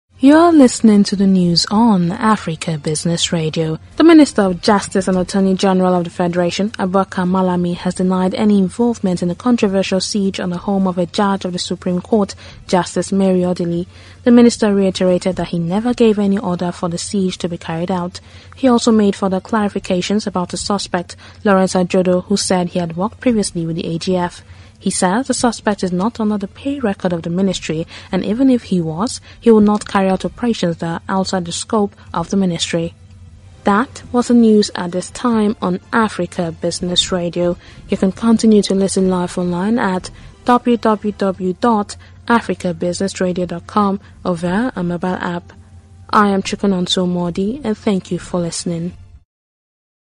Minister of Justice Reiterates Non-Involvement In Siege
The Minister of Justice and Attorney General of the Federation, Abubakar Malami has denied any involvement in the controversial siege on the home of a Judge of the Supreme Court, Justice Mary Odili.
The minister reiterated that he never gave any order for the siege to be carried out.
He also made further clarifications about a suspect – Lawrence Ajodo, who said he had worked previously with the AGF.
He says the suspect is not under the pay record of the ministry and even if he was, he would not carry out operations that are outside the scope of the ministry.